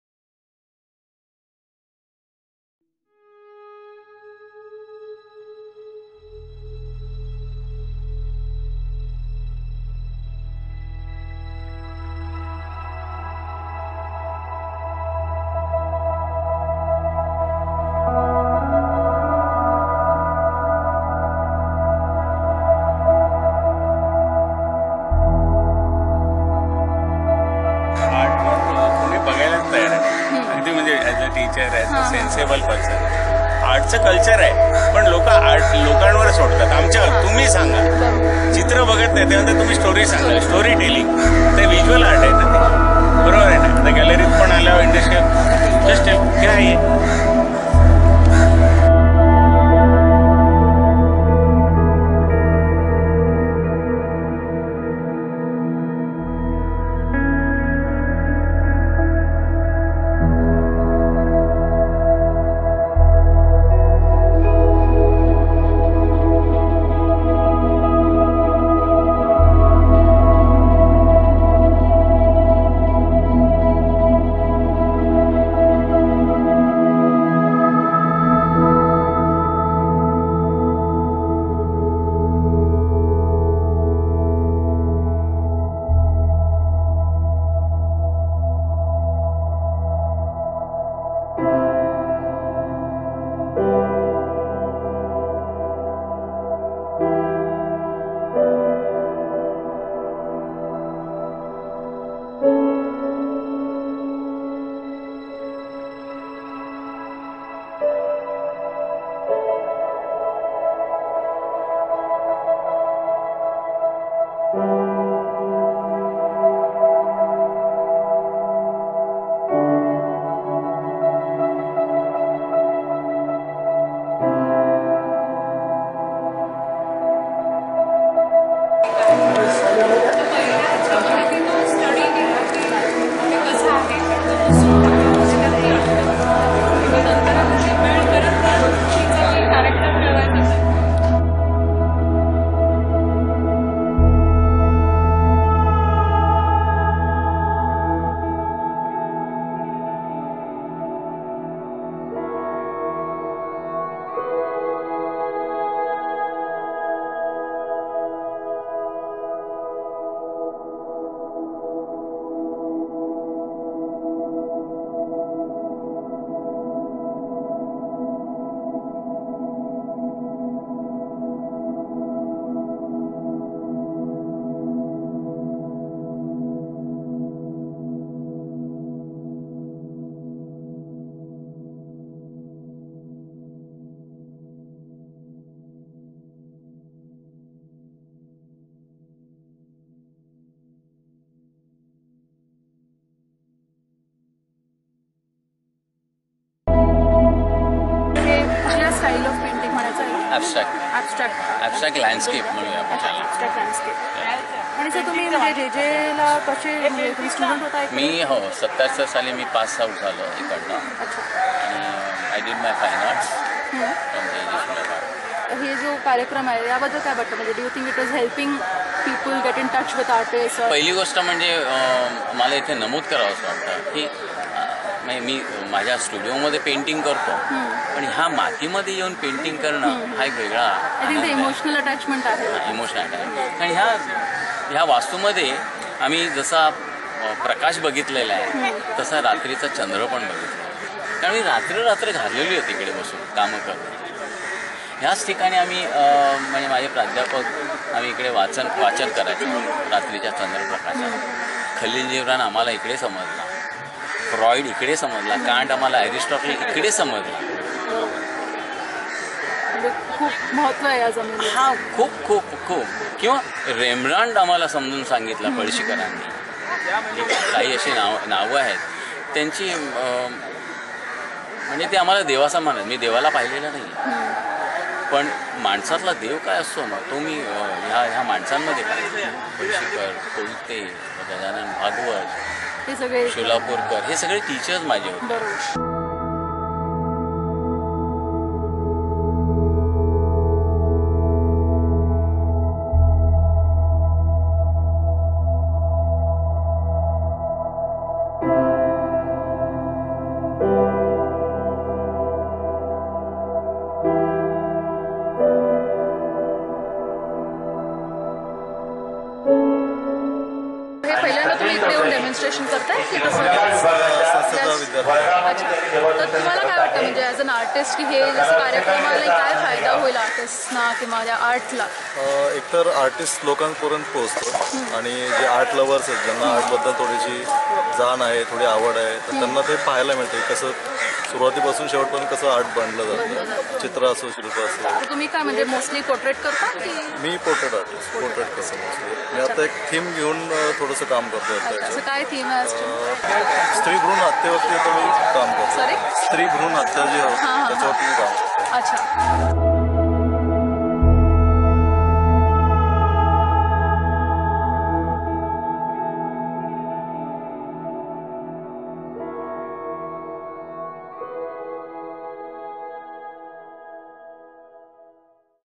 Watch artists make sculptures, paintings live at JJ School Of Arts
The JJ School of Arts and Bombay Art Society's open air art initiative demystified art and bridged the gap between artists and aficionados